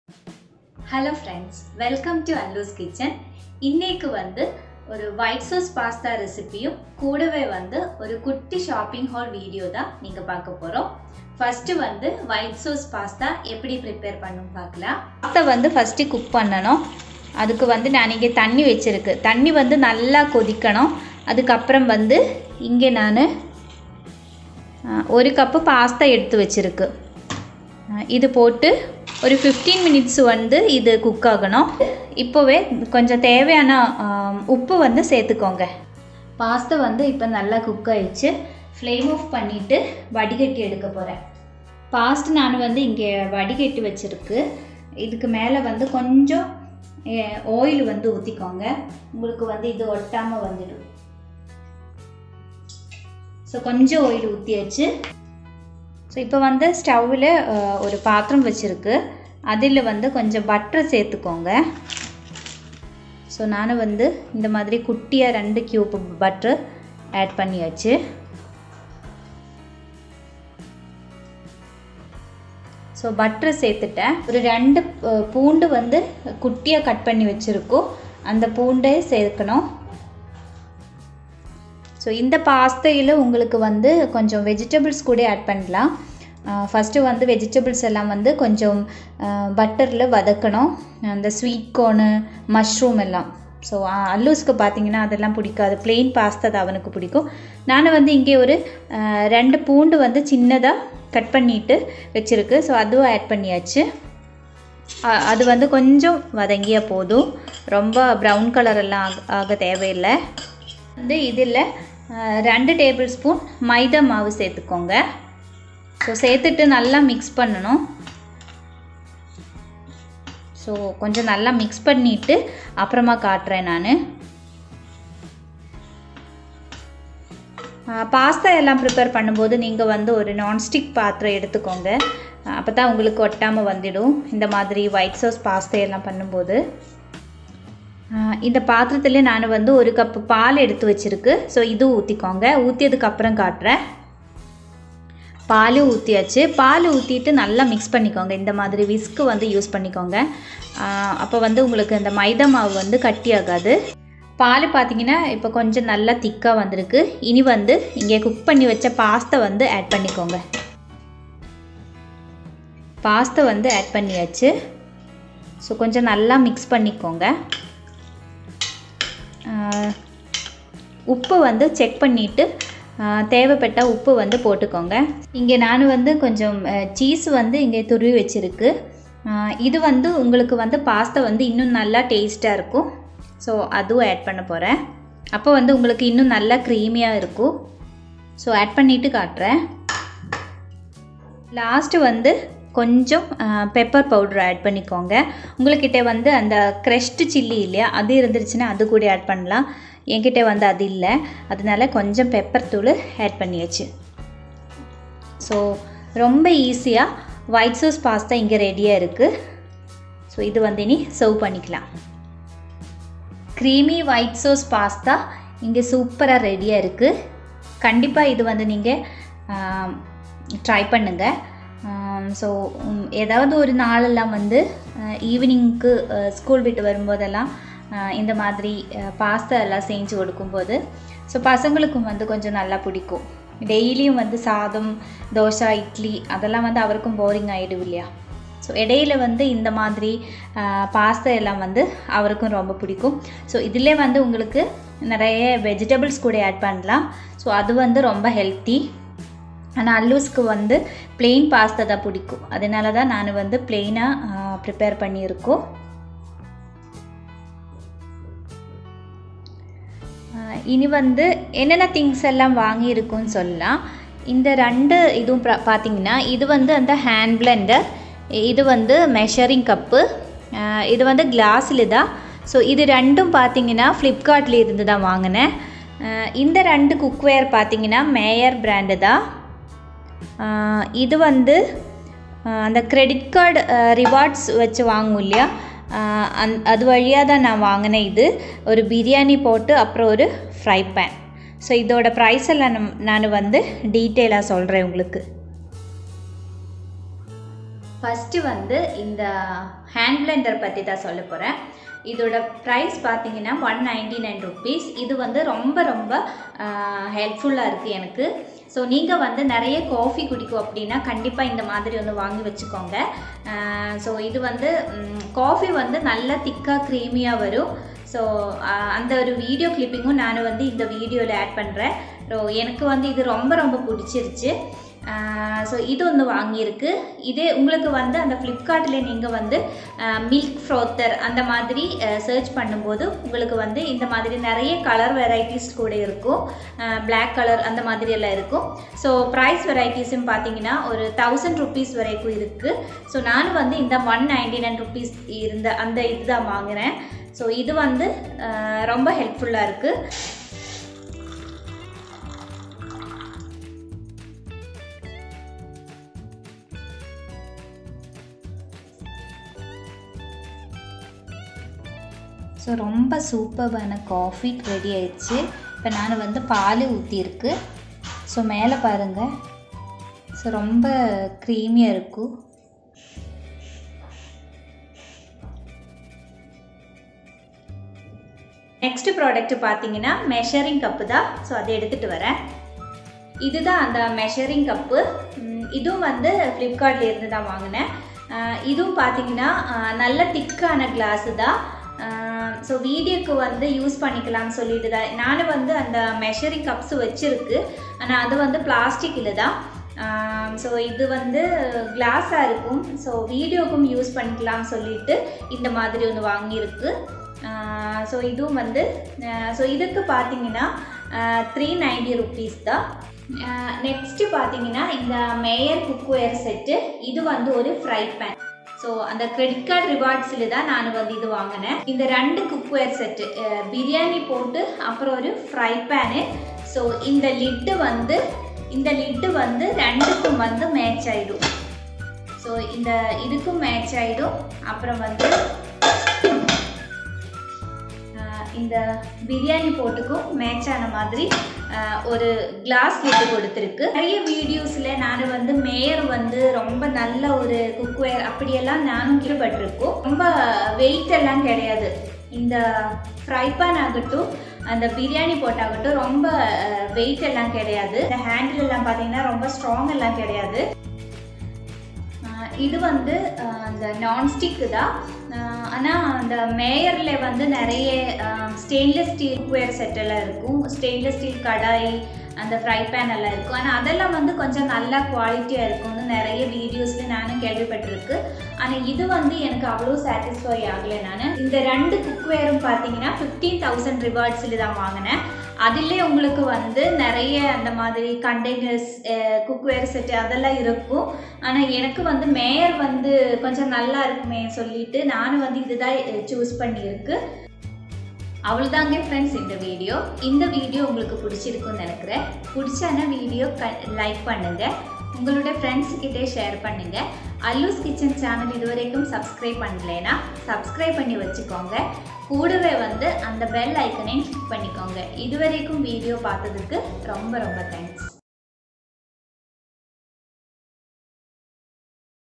White Sauce Pasta --- Shopping Haul[ flipcart, meyer cookware] FREE--Cookware
this video shows my shopping haul and white sauce pasta